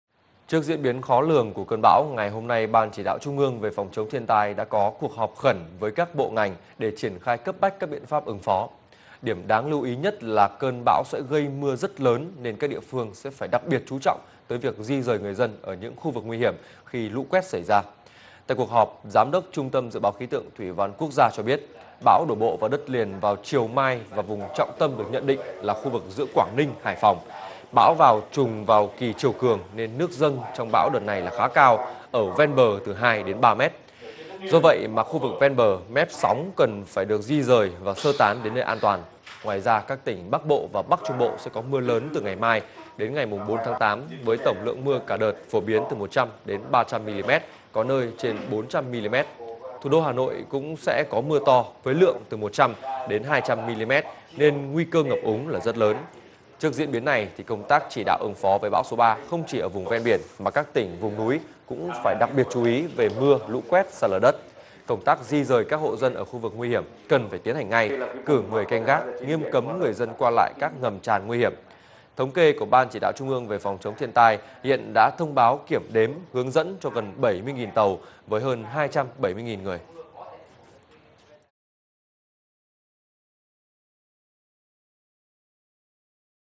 Triển khai phương án ứng phó cơn bão số 3
VTC1 | Ban Chỉ đạo trung ương về Phòng, chống thiên tai đã có đưa ra những phương án nào để ứng phó với con bão số 3?